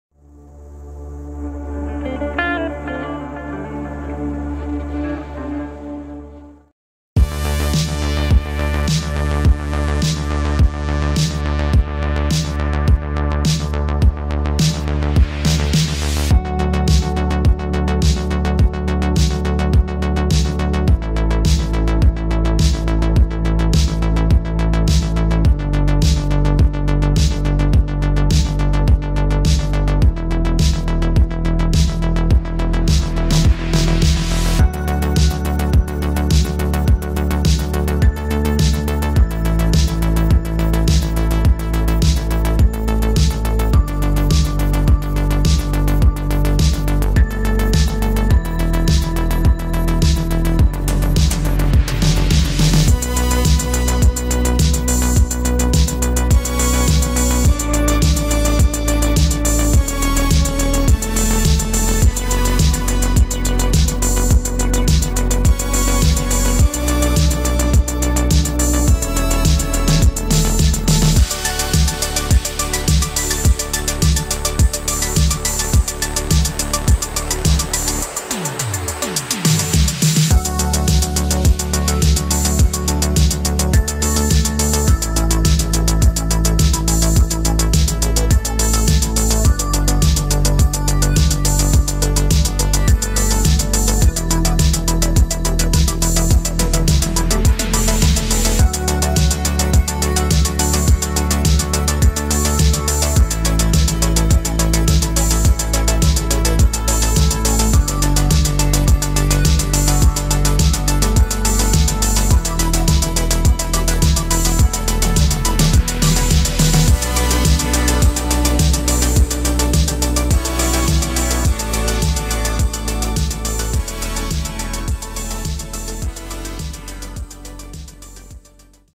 أنطلاق الأشغال بثاني أكبر محطة لتحلية مياه البحر بالجزائر بمنطقة تغرمت ولاية بجاية
NEW BEJAIA DESSALINATION PLANT 300 000 M3 PER DAY